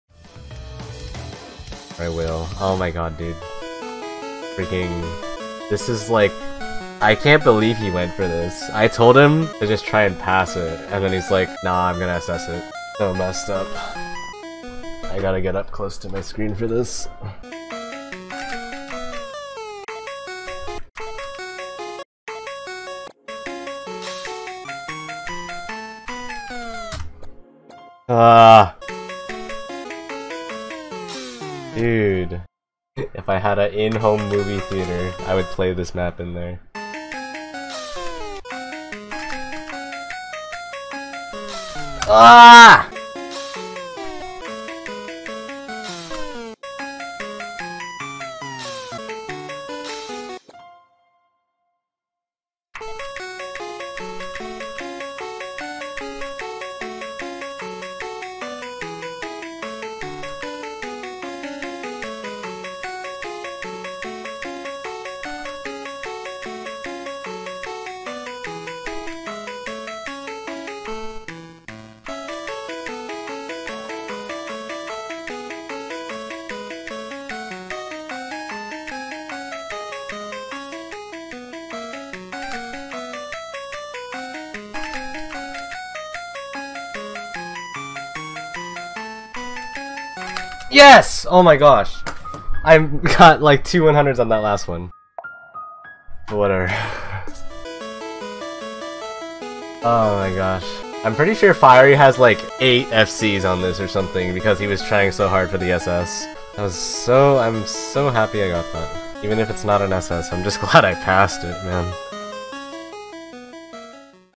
how does he do it...
From my stream on October 5th, 2022.

Wanted to get this after fiery SS'd it after a lot of attempts after I told him to try for it. Hi fiery if you are reading this I am so sorry for making you stay up late that night. This map and many other precision maps have the inherit curse of also being pass maps simply due to HR and low note density making the HP high and strict. To pass this map you basically need to FC but even on top of that you can't miss very many slider ends.